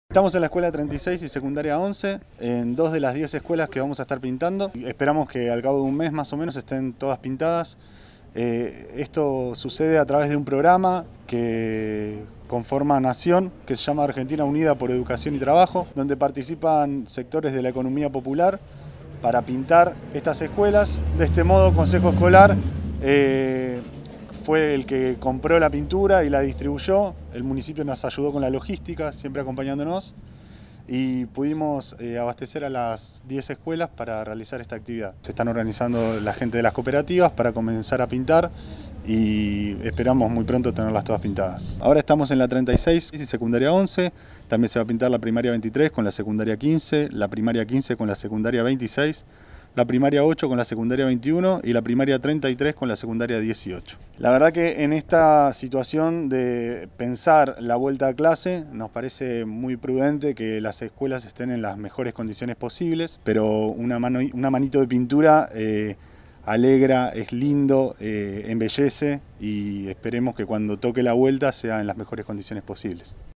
El Municipio de San Fernando colabora con la pintura de 10 Escuelas
Para que alumnos y alumnas puedan tener mejores condiciones cuando se pueda volver a clases, comenzaron las obras de puesta en valor en interiores y exteriores de 10 colegios de San Fernando, mediante el Programa Nacional “Argentina Unida por la Educación y el Trabajo”. La Comuna colabora con el Consejo Escolar y cooperativas barriales que lo realizan en conjunto.

La Agencia de Noticias de la Primera Sección Electoral